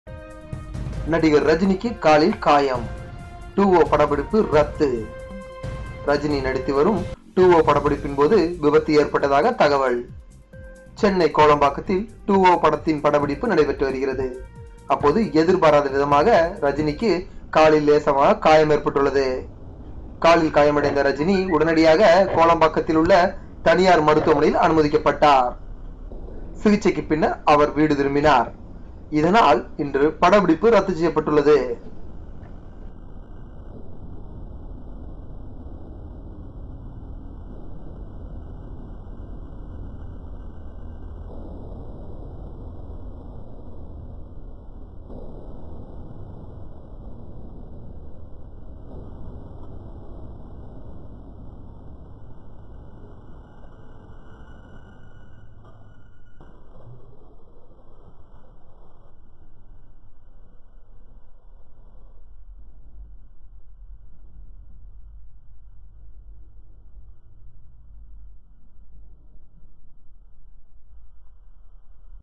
2.0 படப்பிடிப்பின்ந டிகர் ரஜினிகாந்திற்கு காலில் காயம் ஏற்பட்டது
ரஜினியின் 2.0 படப்பிடிப்பின் காலில் காயம் ஏற்பட்டதால் படப்பிடிப்பு ரத்து .

நடிகர் ரஜினிக்கு காலில் காயம் 2.ஓ படப்பிடிப்பு ரத்து ரஜினி நடித்து வரும் 2.ஓ படப்பிடிப்பின் போது விபத்து ஏற்பட்டது.

. சென்னை கேளம்பாக்கத்தில் 2.o படத்தின் படப்பிடிப்பு நடைபெற்றது. அப்போது எதிர்பாரதவிதமாக ரஜினிக்கு காலில் லேசான காயம் ஏற்பட்டது.  காலில் காயம் அடைந்த ரஜினி உடனடியாக கேளம்பாக்கத்தில் உள்ள தனியார் மருத்துவமனையில் அனுமதிக்கப்பட்டார். சிகிச்சைக்கு பின்னர் அவர் வீடு திரும்பினார். இதனால் படப்பிடிப்பு ரத்து செய்யப்பட்டுள்ளது.